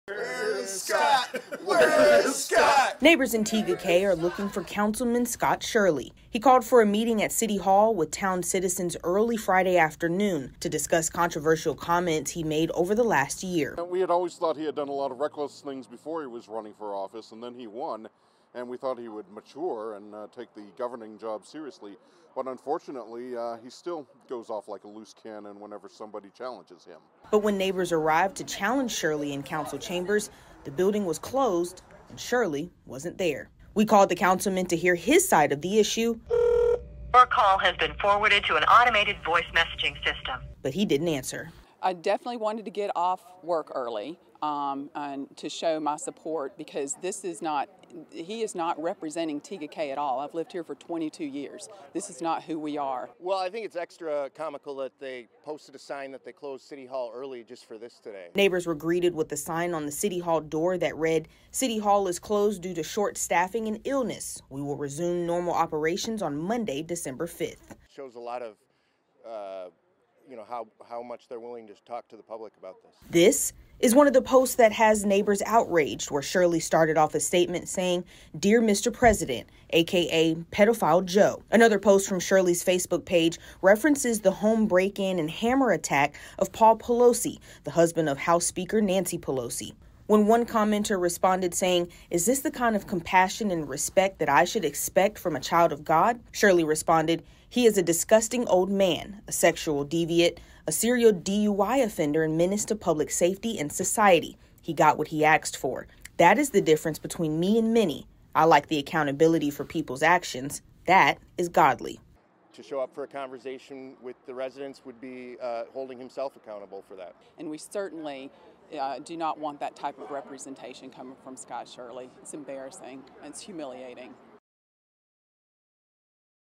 Community upset by Tega Cay councilman's social media posts
Tega Cay residents wanted to meet with councilman Scott Shirley on Friday but found the city hall closed. A few shared their thoughts on his social media posts, and some say they'll come to the next meeting to confront him.